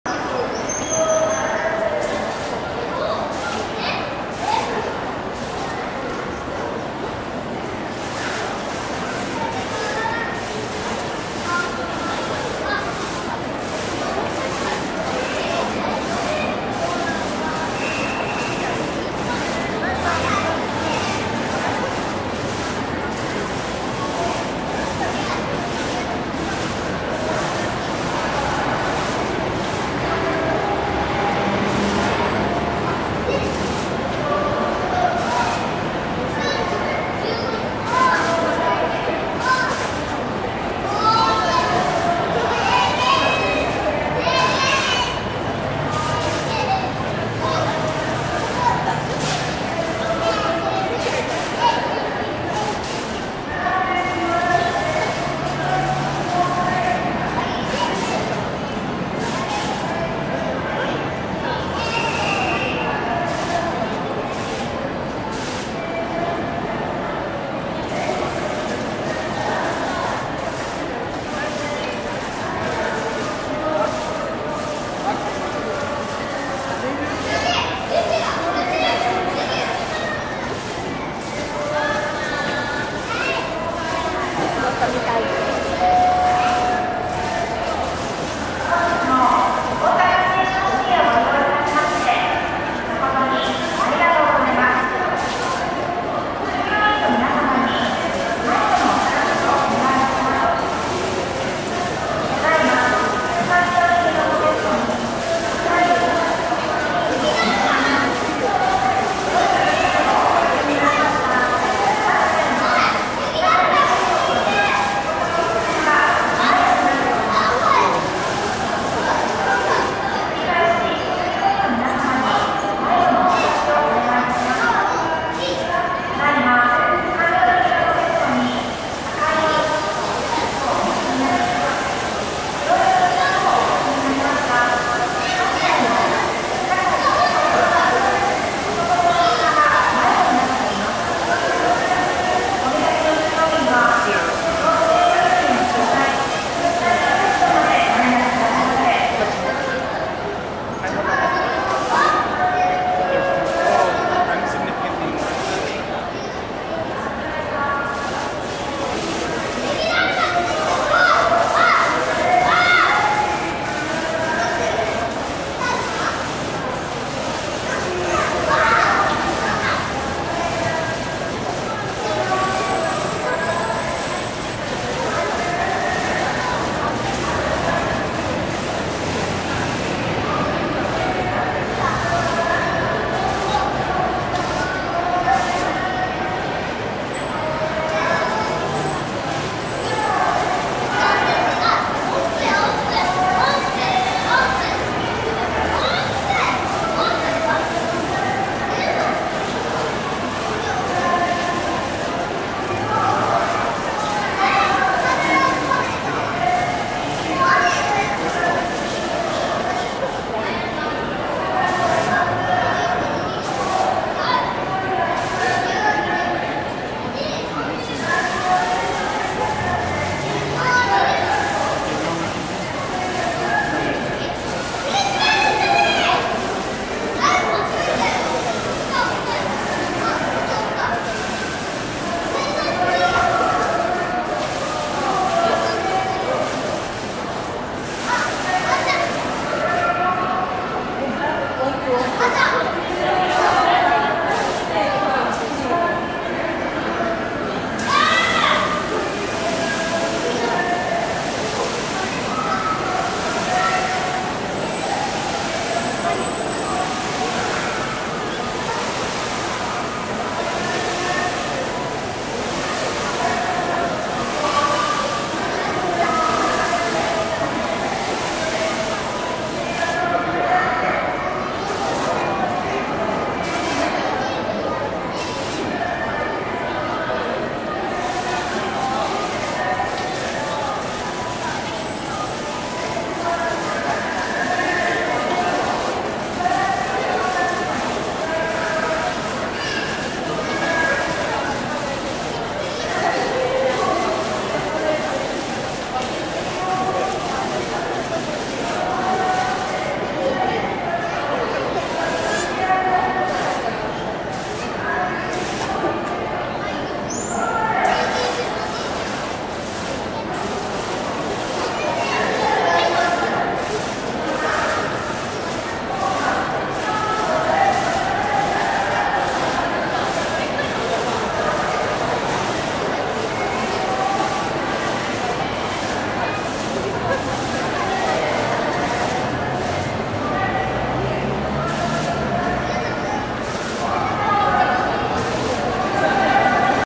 【HD】JR大阪駅・サウスゲートビルの水時計【Stereo】
近鉄京都駅にもある滝アートよりサイズがやや大きい
CAM:GC-PX1(JVC)